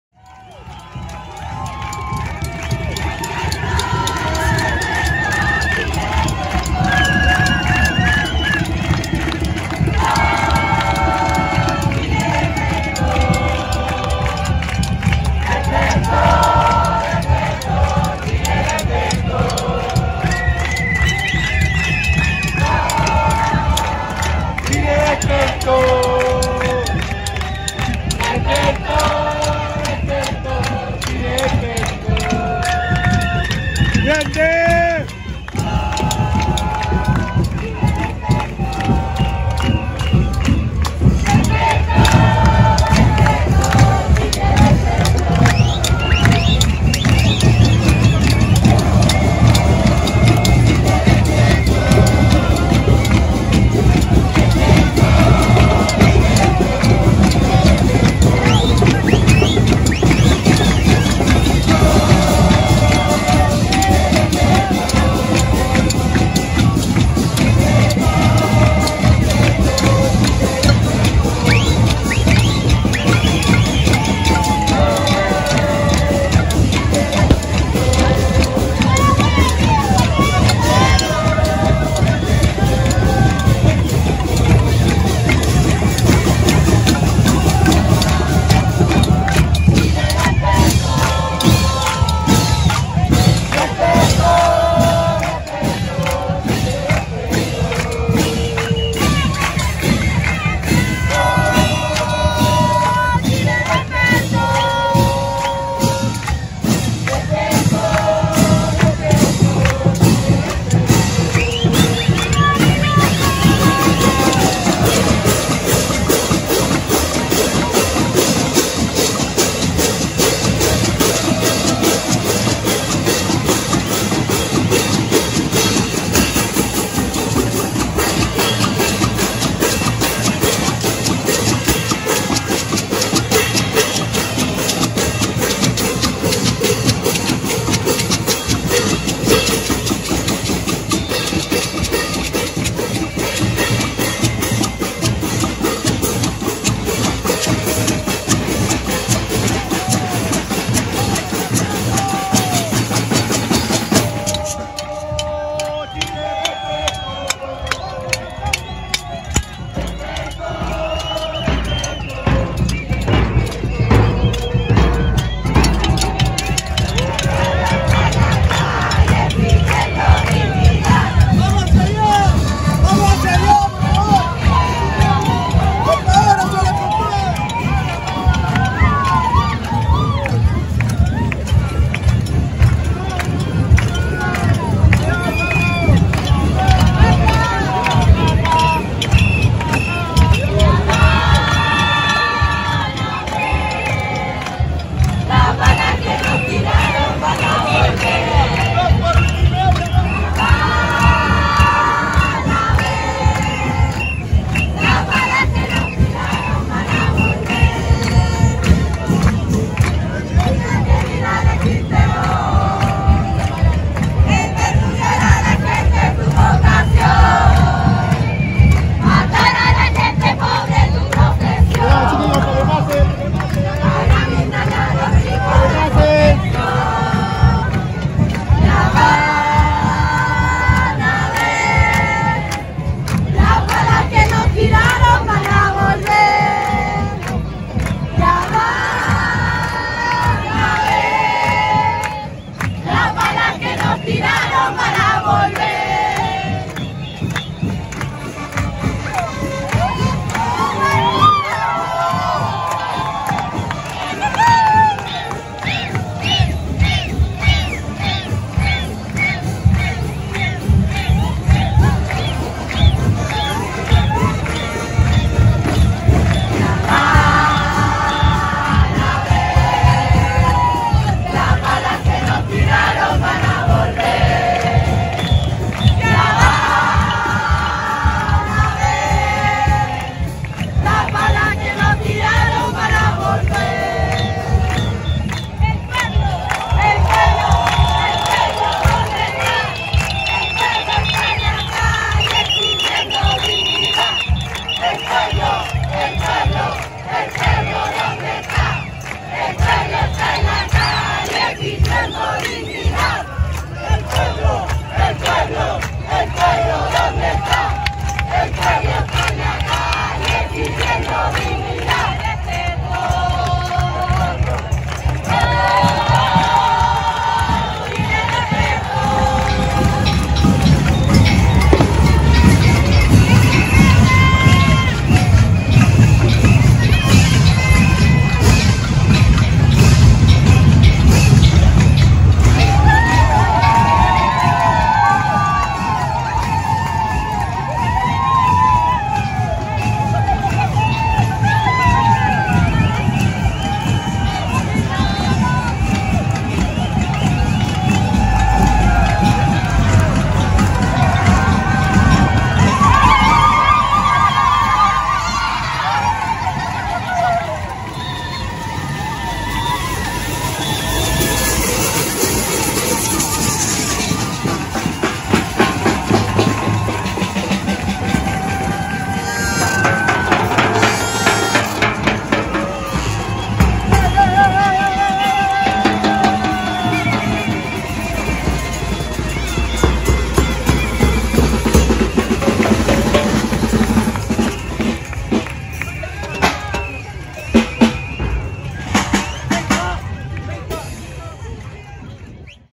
La gran comparsa del pueblo (Plaza de la Dignidad, Diciembre 2019)
La gran comparsa del pueblo /// Día 50 del estallido social /// Viernes 6 de Diciembre de 2019 /// Plaza de la Dignidad /// Santiago /// Chile